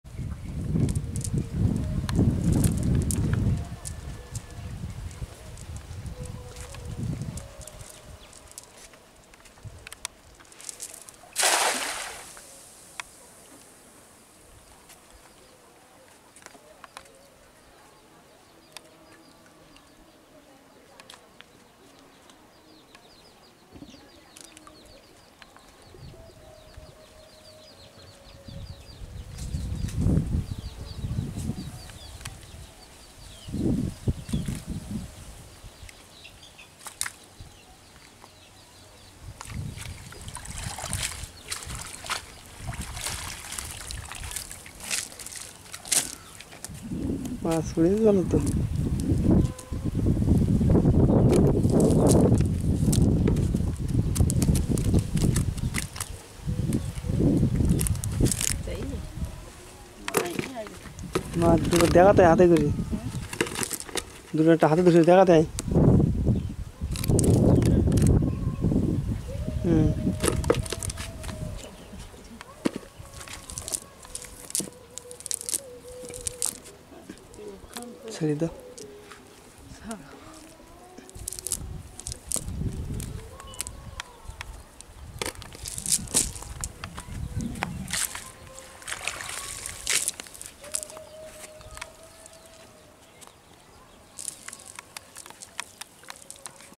Cast Net Fishing | Catching Fish With A Cast Net By Village Women (Part-86) Daily Village Fishing
Daily Village Fishing